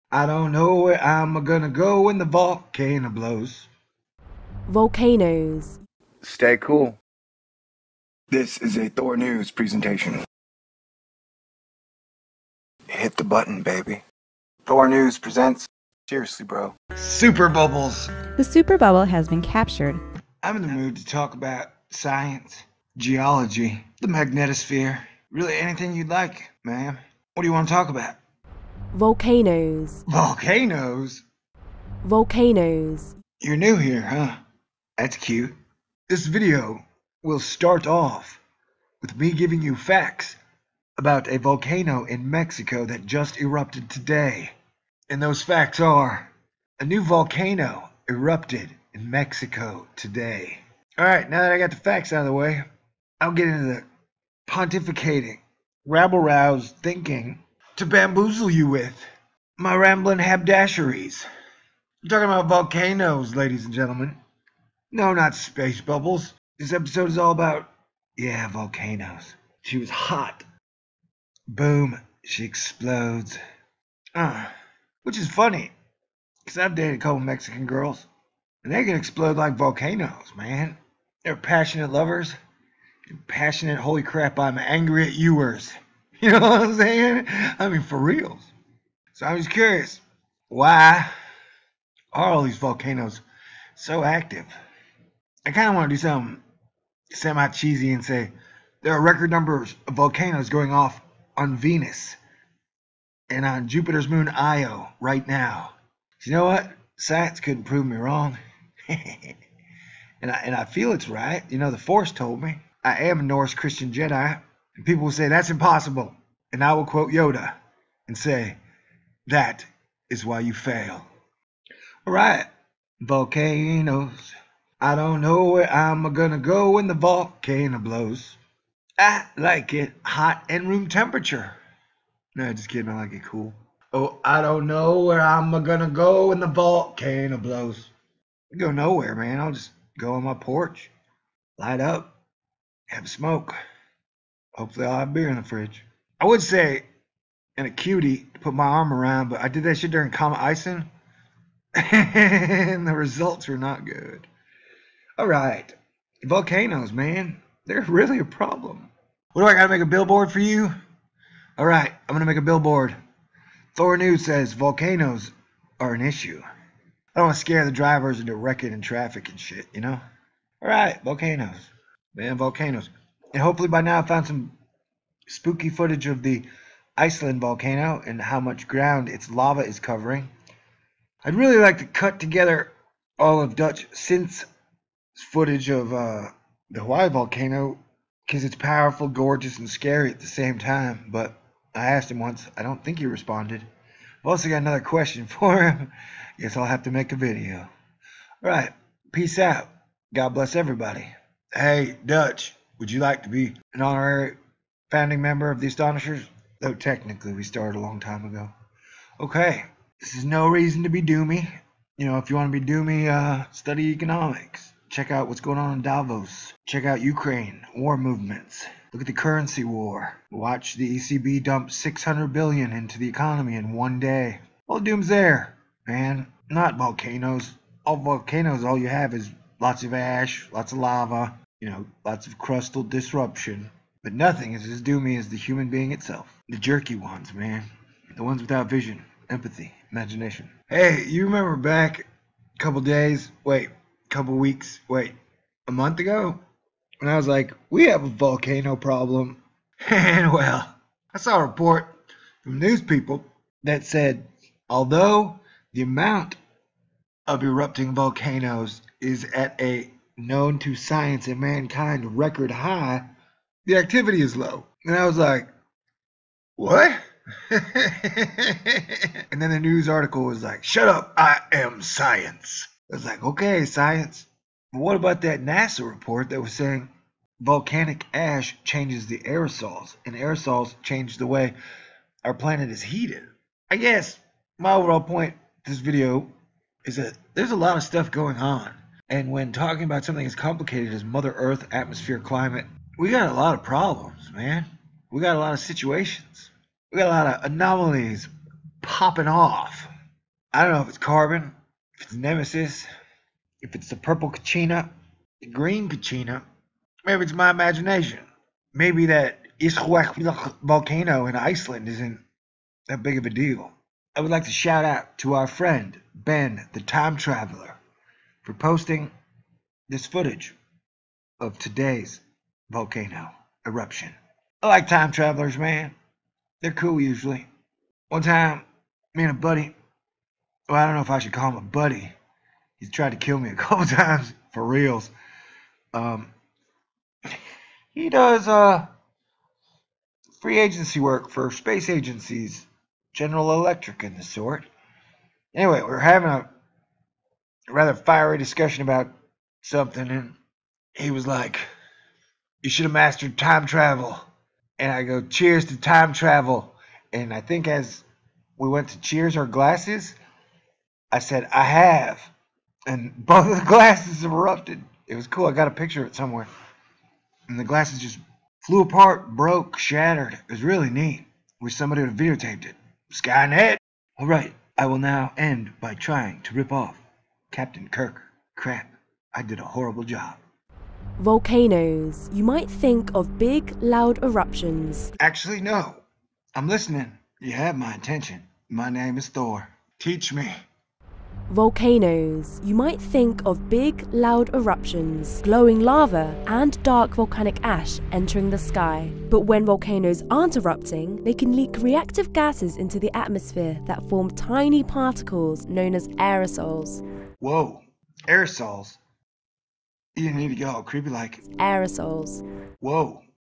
Mexican Eruption: We have a Volcano Problem pt. 2 - Fire, Ice & Ash
The Colima Volcano erupted in Mexico this morning. The Lava field in  Iceland's Bardarbunga volcano is larger than Manhattan and civilized parts of Hawaii are turning into a lava mess.

Thanks to Ben Time Traveler for his video.

I recommend watching his video also. The Music is perfect.

Crazy days, indeed.

T